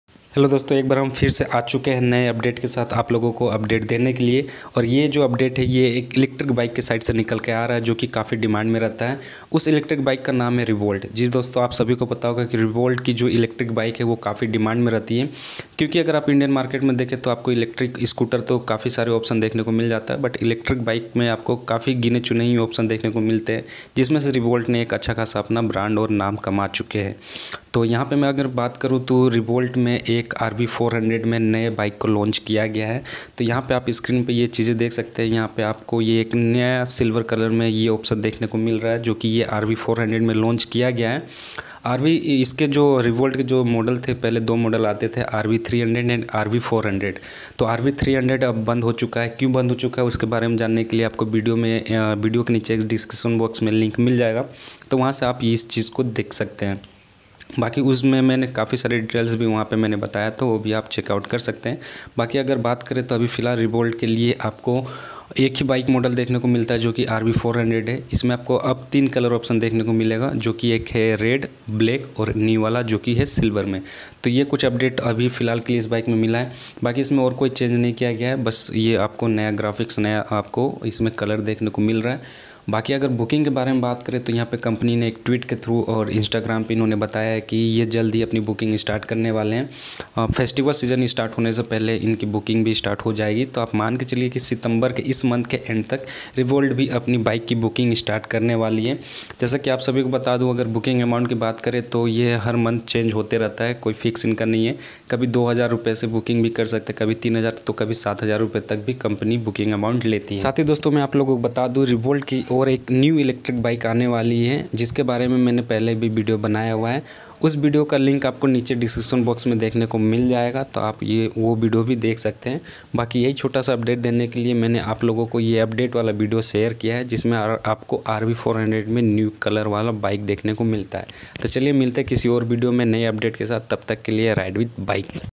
Finally REVOLT All New RV400 Launched With New Colour |RV300 Discontinue Before 2022 |Revolt Booking
Revolt All VVI Video Link :-